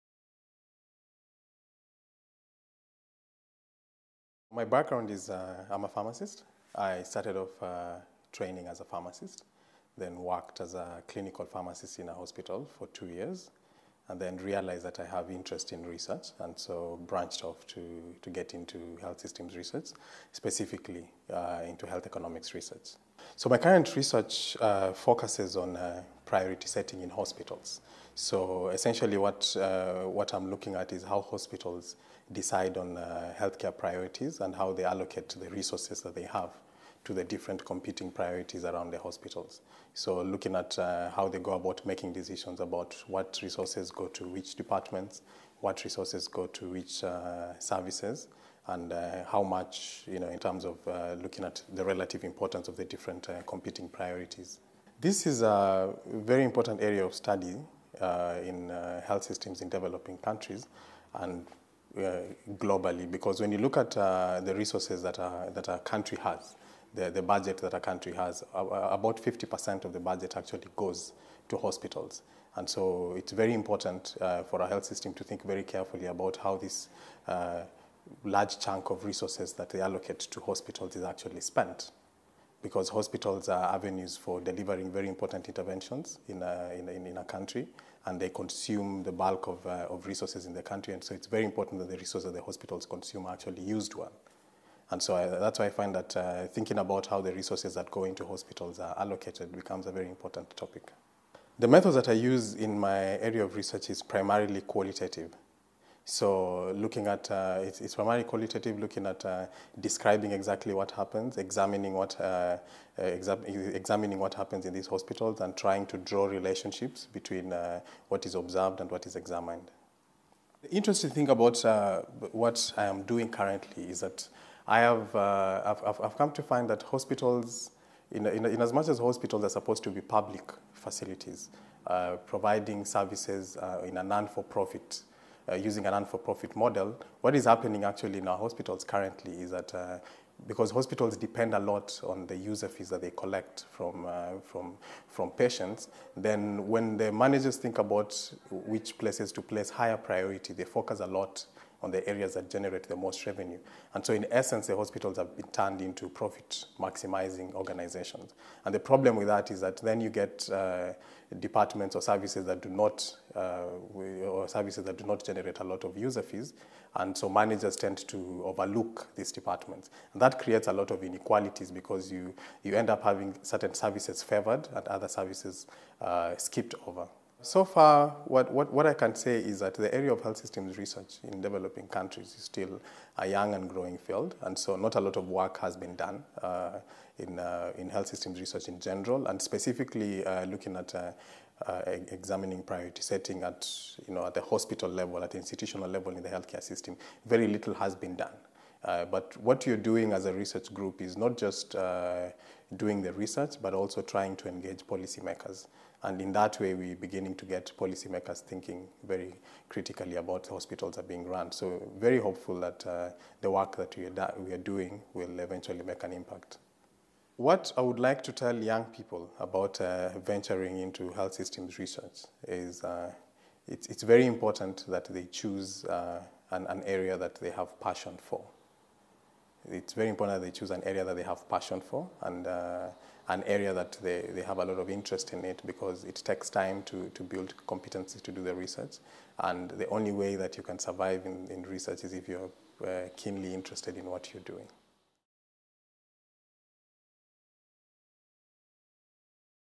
Interview with Edwine Barasa: Economics of hospitals
Edwine Barasa describes and evaluates the priority setting practices in county hospitals in Kenya.